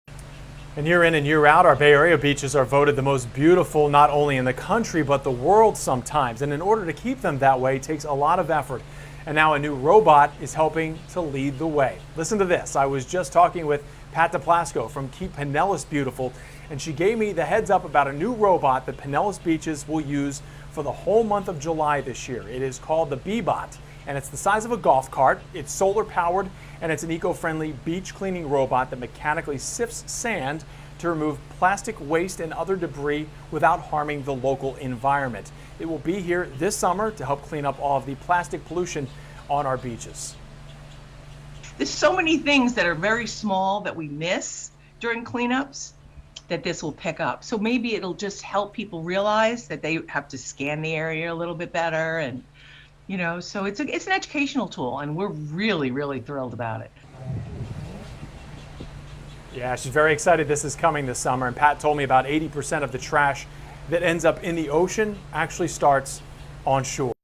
Beach cleaning robot will help keep Pinellas beaches beautiful this summer
The “Bebot” is a solar-powered and eco-friendly beach cleaning robot that will help keep Pinellas beaches clean the whole month of July.